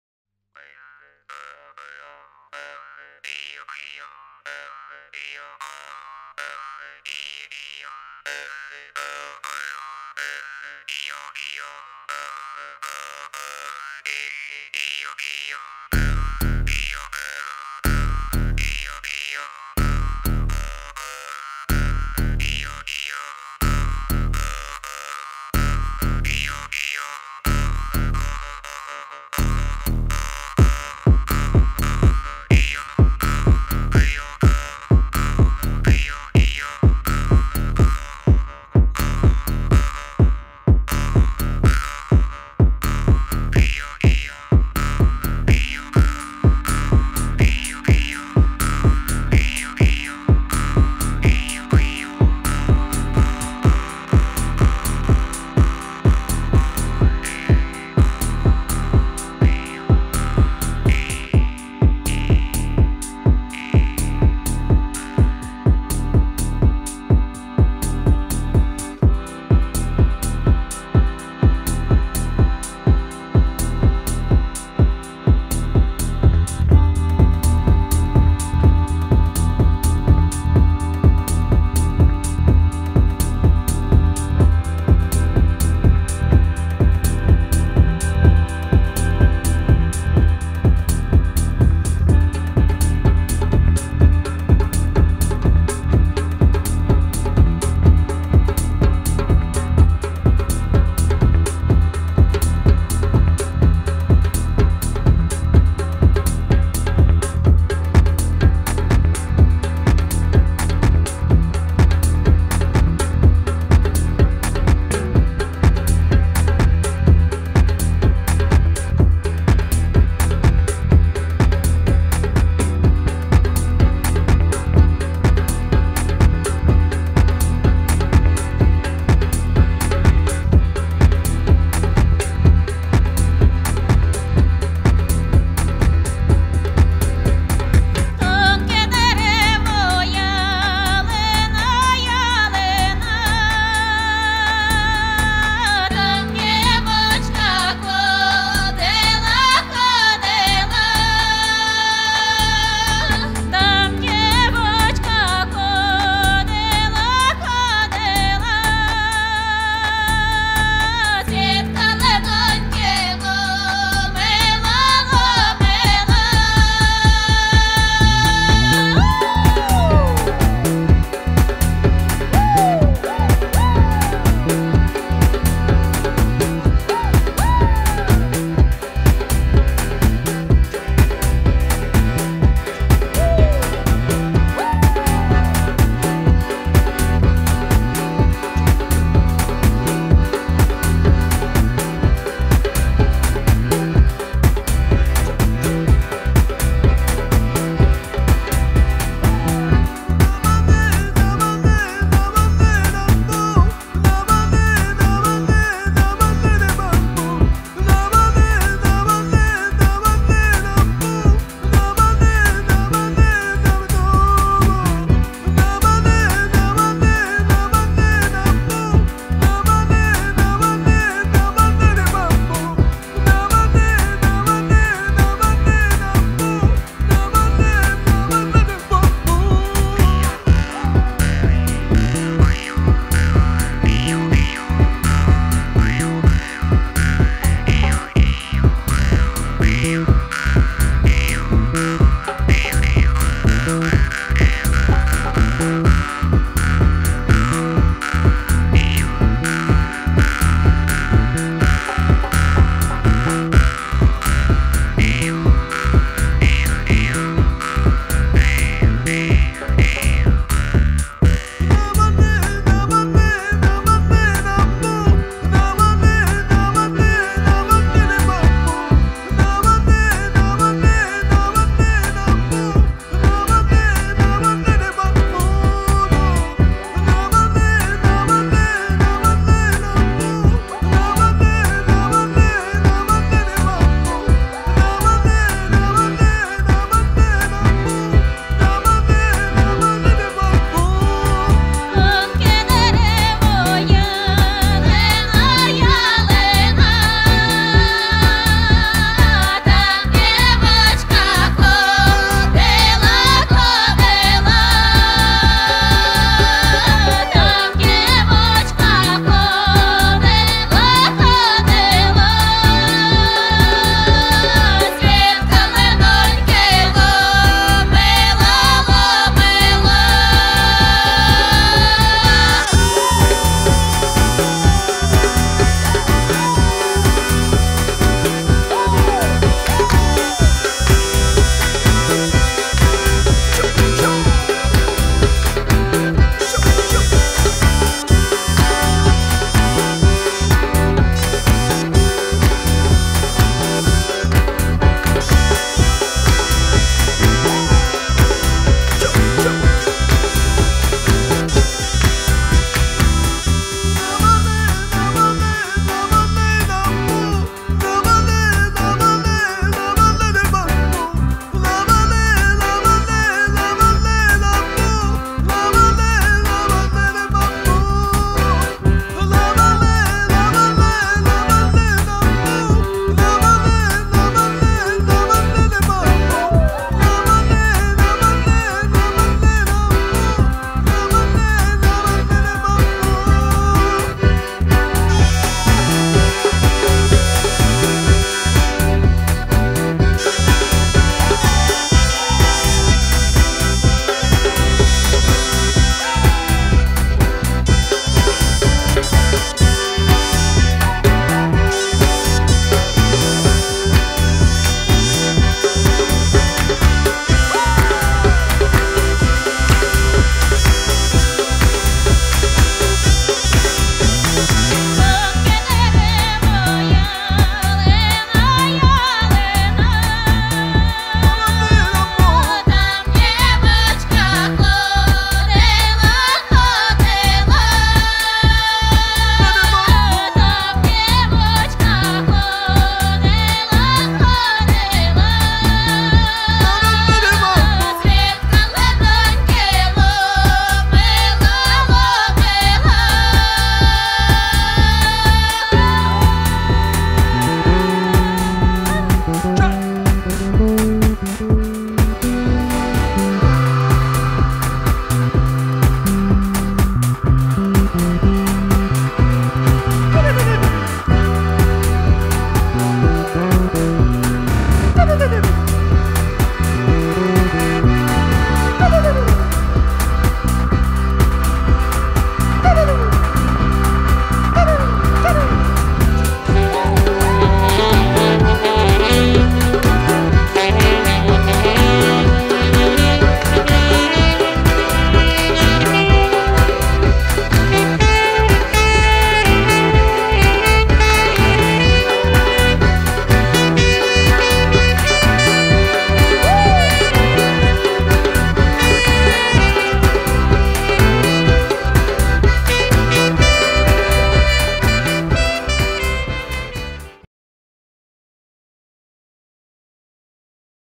АртПоле 2012 - День 2
13 липня другий день фестивалю проходив ось так...

Created by (С) : Olga Baranova, Andrey Barchuk